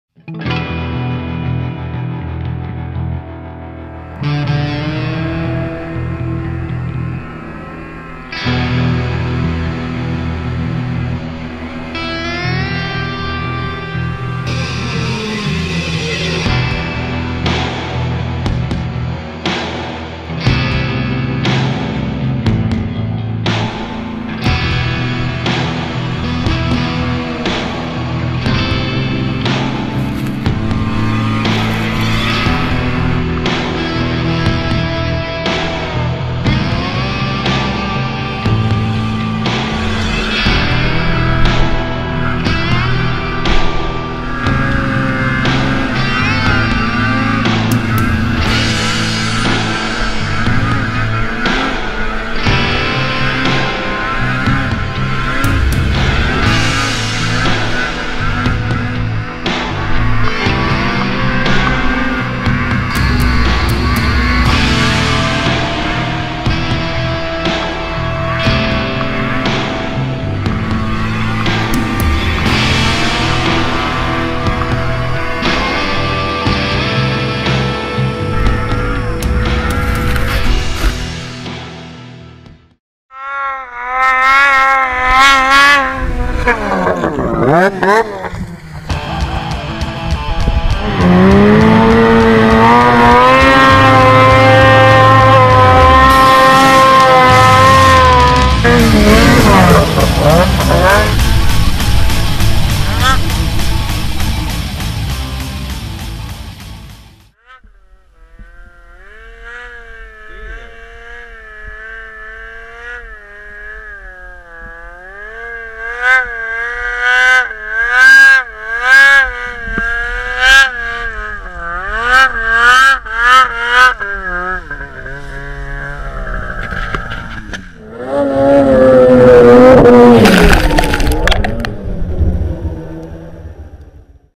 Pro RMK Freeride: Taylor Gensisky WATCH IN 1080HD
Footage spanning 2 seasons of sledding 13/14-14/15 ranging from -30 temps in Manitoba to +5 in BC.

Sled: 2014 Polaris 800 Pro RMK 155, Exit X2 clicker shocks, Jaws race can, Custom wrap by 139 designs.

Rider: Taylor Gensisky